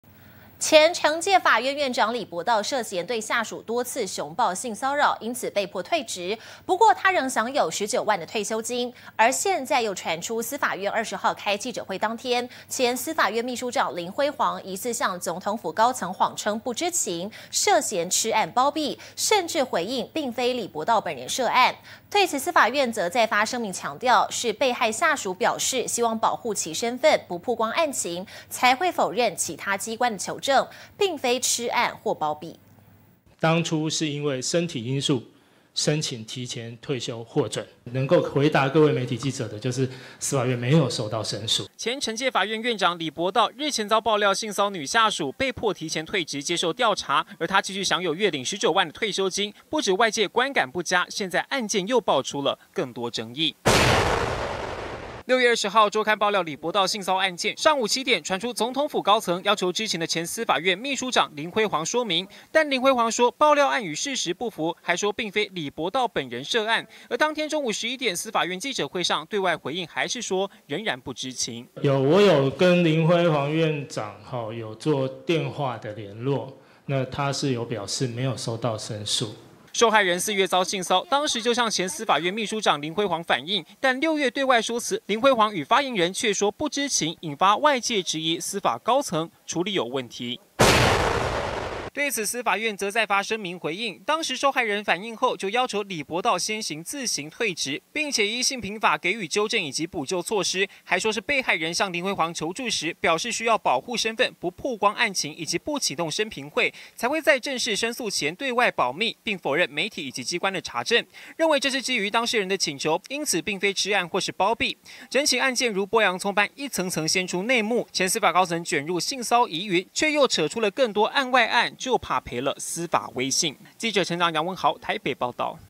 前懲戒法院院長性騷案 知情官員遭疑欺瞞案情
前懲戒法院院長李伯道，涉嫌對下屬多次熊抱性騷擾，因此被迫退職，不過他仍享有19萬的退休金，而現在又傳出，司法院20日開記者會當天，前司法院秘書長林輝煌，疑似向總統府高層謊稱不知情，涉嫌吃案、包庇，甚至回應並非李伯道本人涉案；對此司法院則再發聲明強調，是被害下屬表示希望保護其身分、不曝光案清，才會否認其他機關的求證，並非吃案或是包庇。